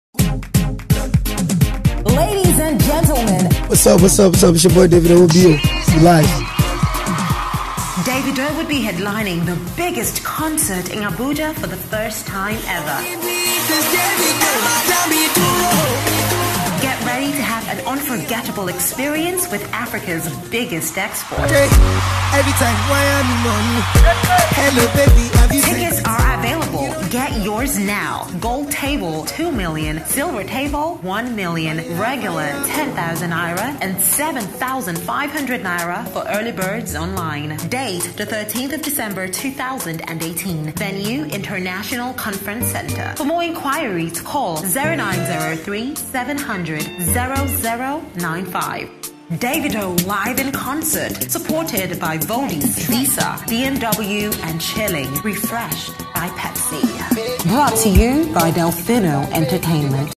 Davido first ever concert in ABUJA DEC 13TH
ABUJA DEC 13TH! FIRST EVER CONCERT IN ABUJA! 🔥🔥🔥🔥🎉🎉🎉 @delphinoentertainment @babzii_chilling